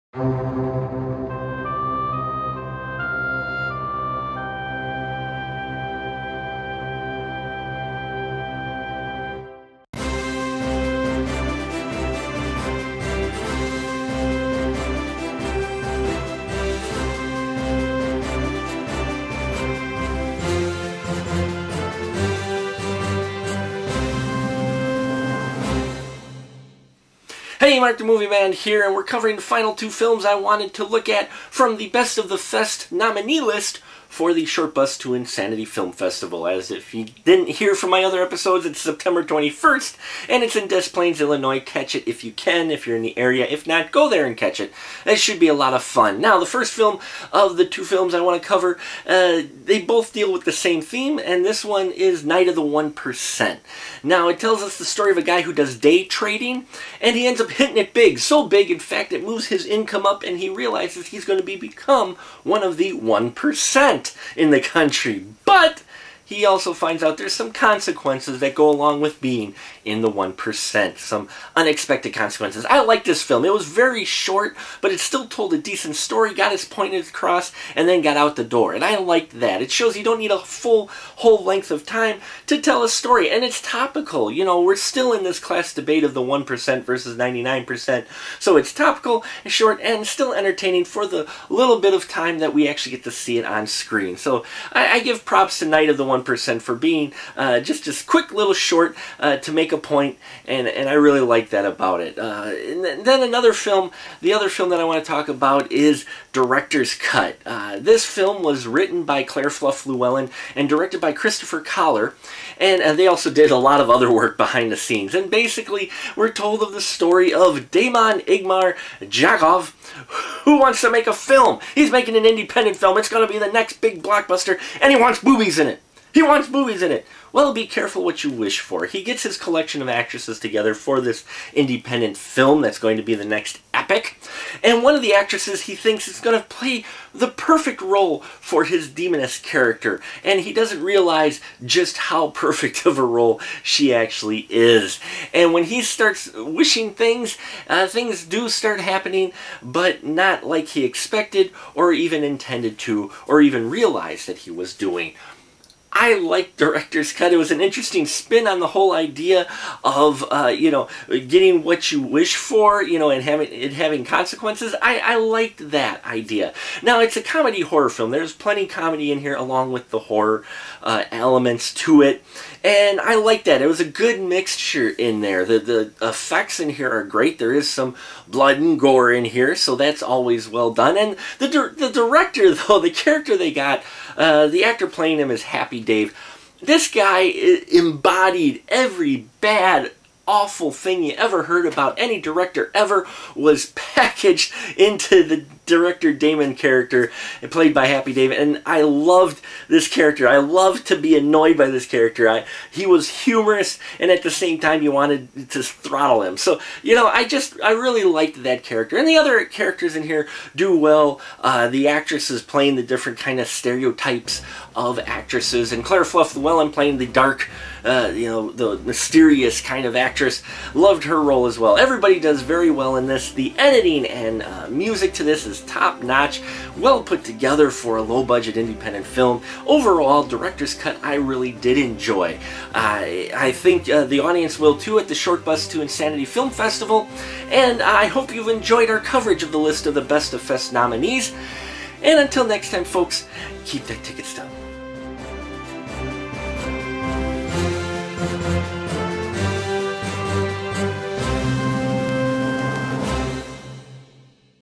Night of the 1% and Director's Cut review on The Final Cut
In our final episode of our coverage for the Short Bus to Insanity Film Festival Best of the Fest Nominees, we have two films with similar themes. Check out our review of  Night of the 1% and Directors Cut.